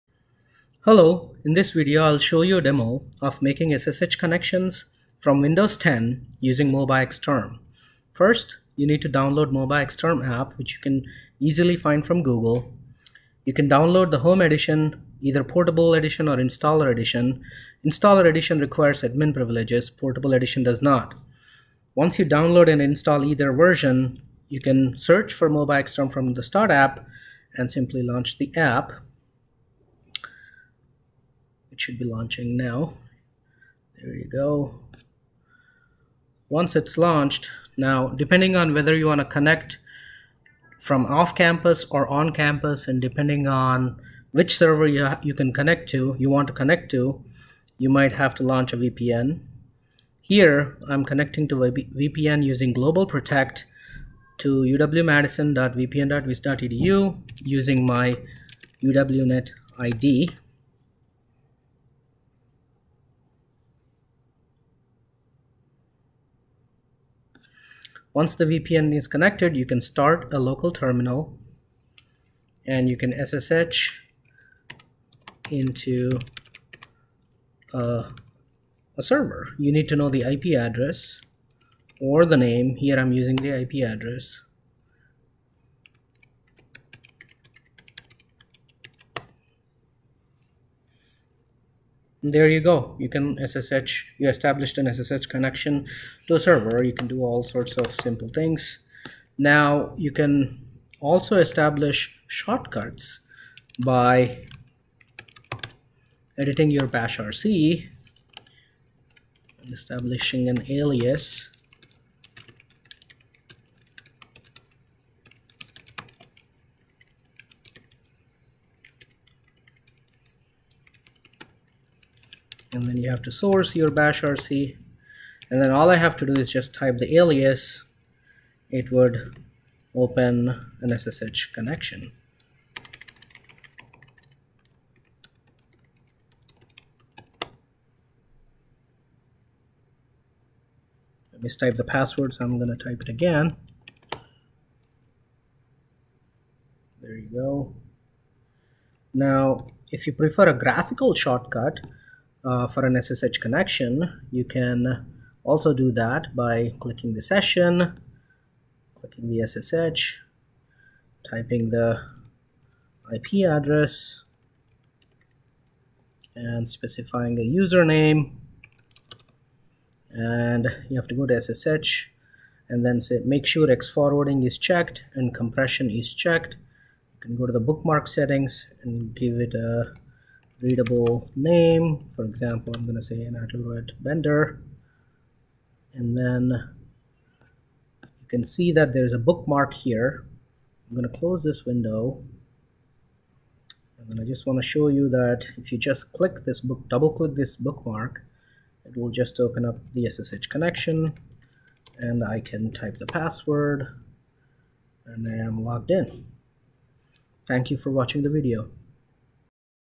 SSH Connections from Windows 10 using MobaXterm
Demo of making SSH connections from Windows 10 using MobaXterm.